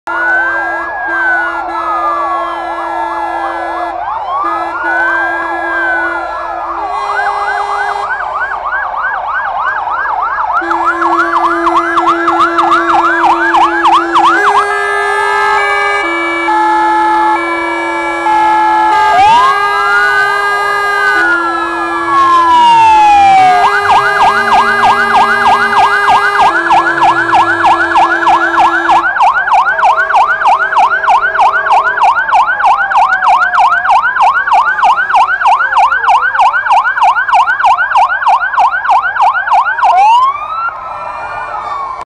341[R]40 GBA-Rt Scania P360/ ISS - JRG Krosno POKAZ SYGNALIZACJI
Pokaz sygnalizacji na samochodzie Scania P360 341[R]40 z JRG Krosno. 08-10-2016 charytatywna akcja dla dh Łukasza organizowana przez portal Krosno112.pl